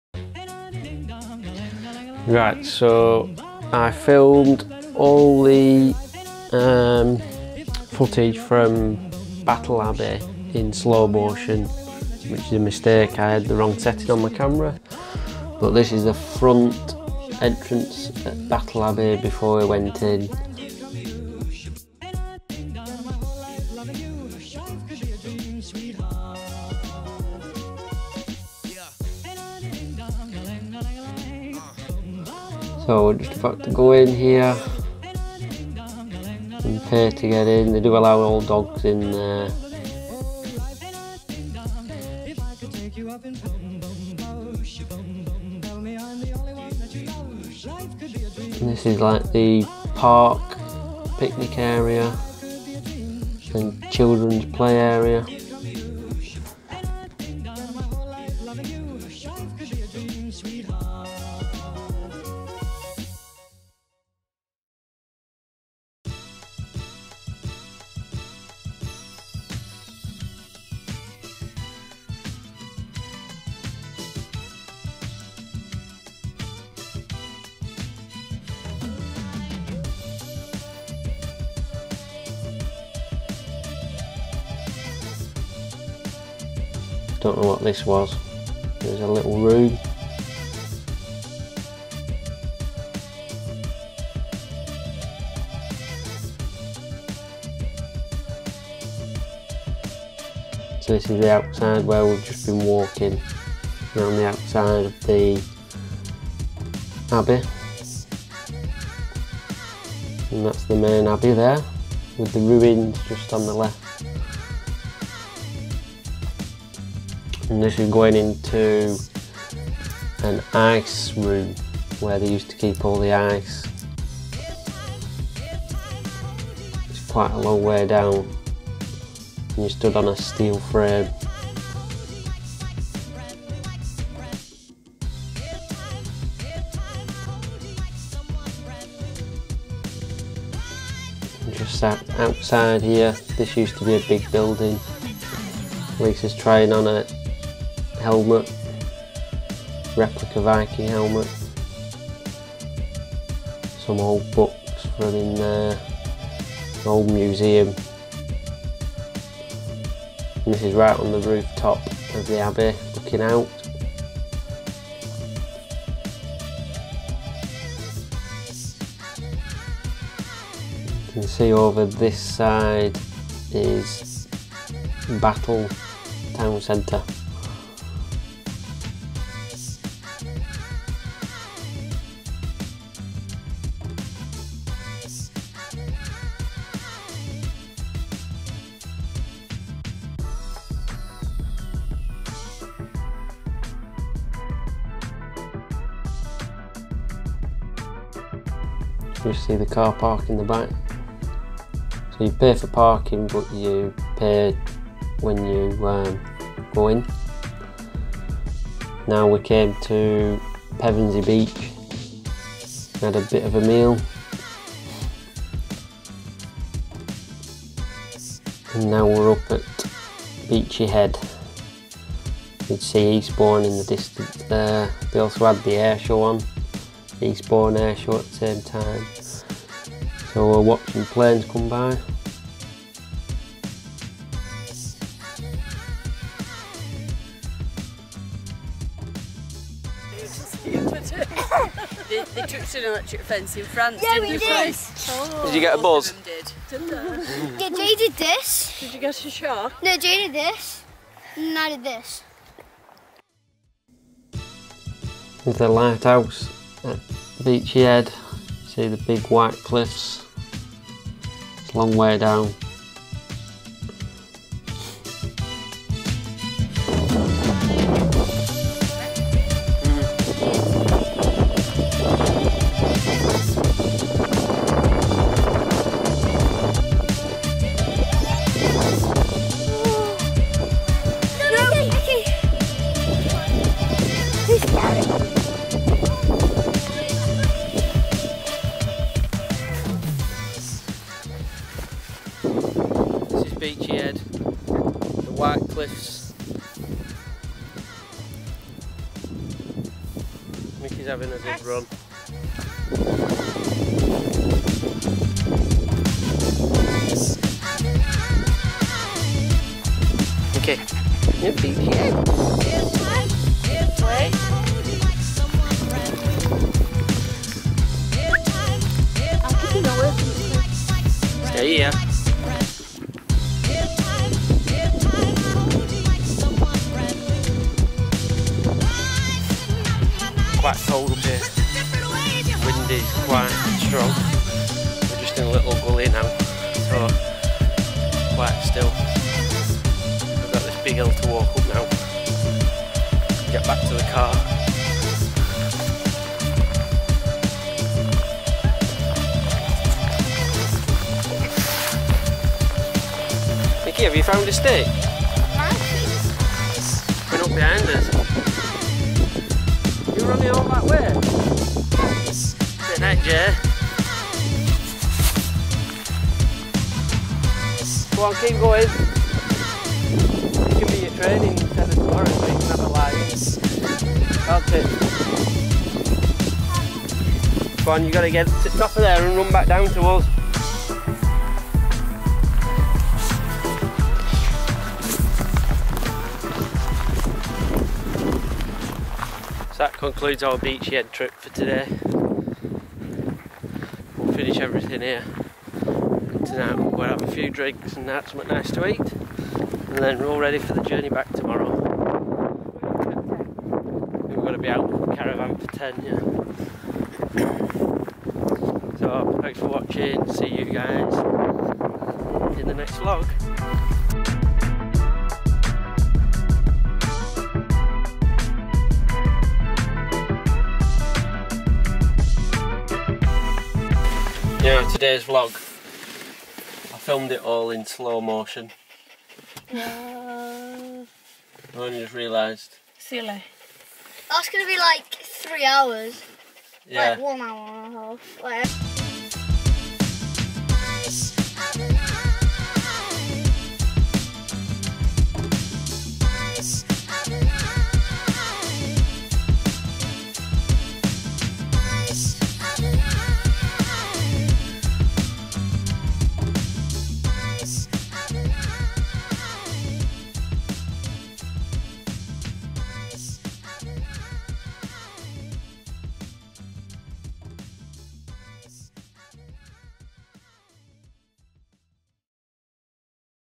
Battle Abbey & Beachy Head Eastbourne Airshow Vlog #015[4k]
We did a Tour of Battle Abbey in Battle East Sussex.  We then went to Beachy Head to a walk and to watch the Eastboune Airshow.  Airborne 2017

My Vlogging Camera Gear like Casey Neistat;
Sony A6300
Cannon G7X Mark 2
Rode Video Mic
Gorilla pod tripod